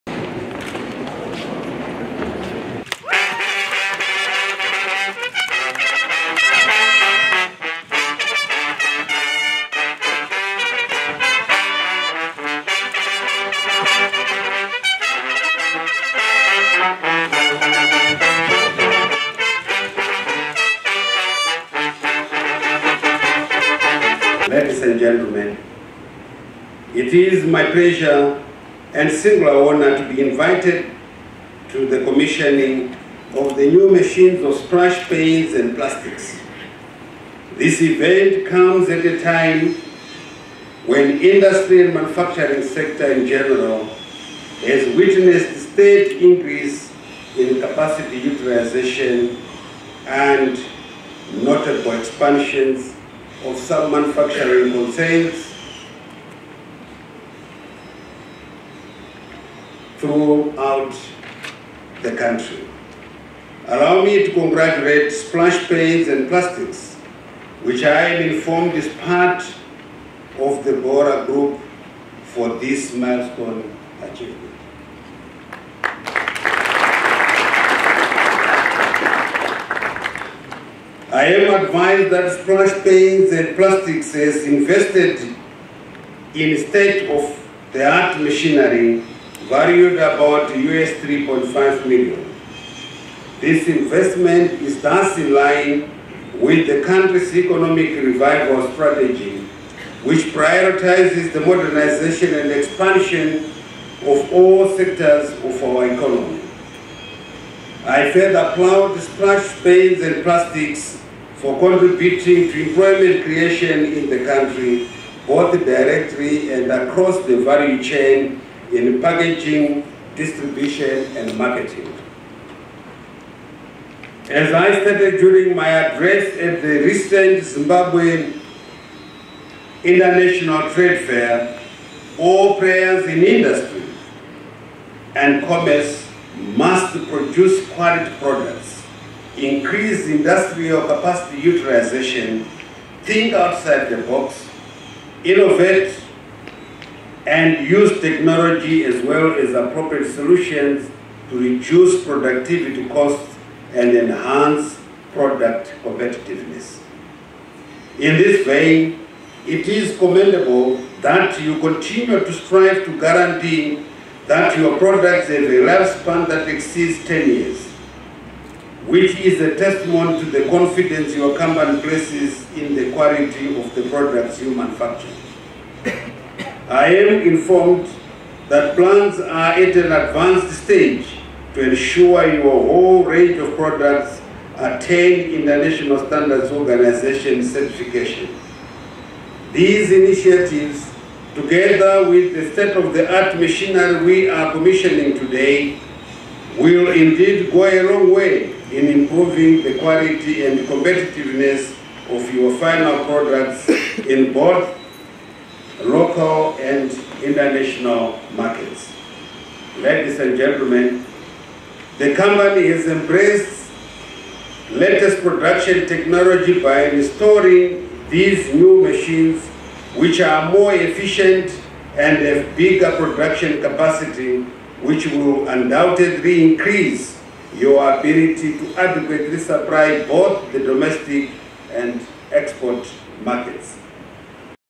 Chiwenga opens Splash paints’s new plant
Vice President Constantino Chiwenga graces the official commissioning of the Splash paints’s new plant in Workington industrial area.